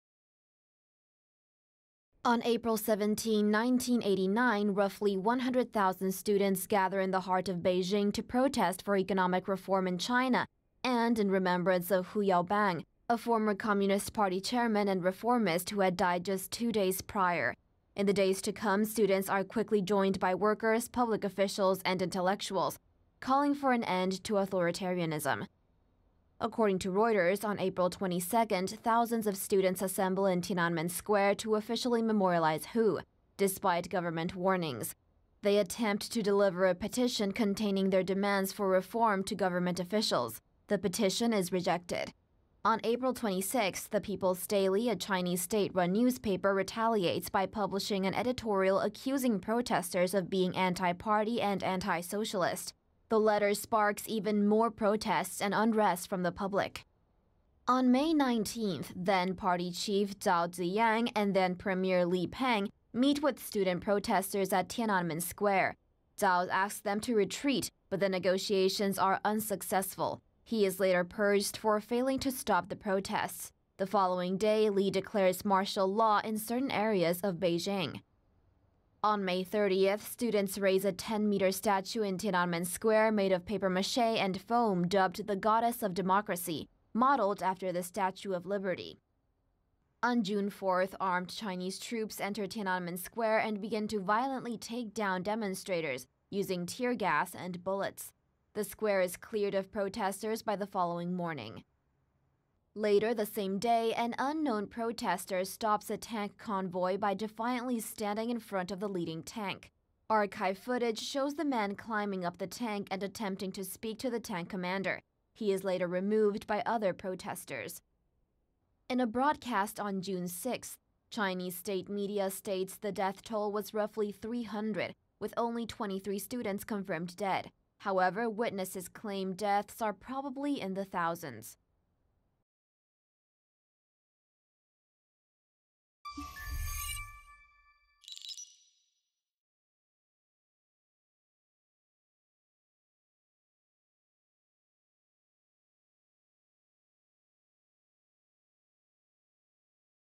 Tiananmen Square: 30 years on
Thirty years ago peaceful protests for democratic reform quickly turned violent as Chinese armed forces turned their weapons against unarmed demonstrators.

The confrontation between military and unarmed civilians cost the lives of many, with the death toll estimated to range from several hundred to thousands. This is a timeline of what led to the Tiananmen Square massacre.

RUNDOWN SHOWS:
1. Protestors take to the streets of Beijing
2. Students' petition is rejected; CCP publishes letter against protestors
3. Zhao Ziyang purged after failing to negotiate with students
4. Students build Goddess of Democracy statue
5. Chinese troops violently remove protestors from Tiananmen Square
6. Unknown man faces line of tankers
7. Unconfirmed deaths

VOICEOVER (in English):

"On April 17, 1989, roughly 100,000 students gather in the heart of Beijing to protest for economic reform in China and in remembrance of Hu Yaobang, a former communist party chairman and reformist who had died just two days prior."

"In the days to come, students are quickly joined by workers, public officials, and intellectuals, calling for an end to authoritarianism.

"According to Reuters, on April 22, thousands of students assemble in Tiananmen Square to officially memorialize Hu, despite government warnings. They attempt to deliver a petition containing their demands for reform to government officials. The petition is rejected."

"On April 26, the People's Daily, a Chinese state-run newspaper, retaliates by publishing an editorial accusing protestors of being anti-party and anti-socialist. The letter sparks even more protests and unrest from the public."

"On May 19, then-party chief, Zhao Ziyang, and then-premier, Li Peng, meet with student protestors at Tiananmen Square. Zhao asks them to retreat but the negotiations are unsuccessful. He is later purged for failing to stop the protests. The following day Li declares martial law in certain areas of Beijing."

"On May 30th, students raise a 10-meter statue in Tiananmen Square made of paper-mache and foam dubbed the "Goddess of Democracy" modeled after the Statue of Liberty."

"On June 4th armed Chinese troops enter Tiananmen Square and begin to violently take down demonstrators using tear gas and bullets. The square is cleared of protestors by the following morning."

"Later the same day an unknown protestor stops a tank convoy by defiantly standing in front of the leading tank.  Archive footage shows the man climbing up the tank and attempting to speak to the tank commander. He is later removed by other protestors."

"In a broadcast on June 6th, Chinese State media states the death toll was roughly 300 with only 23 students confirmed dead. However, witnesses claim deaths are probably in the thousands."

SOURCES:
Reuters, CNN, BBC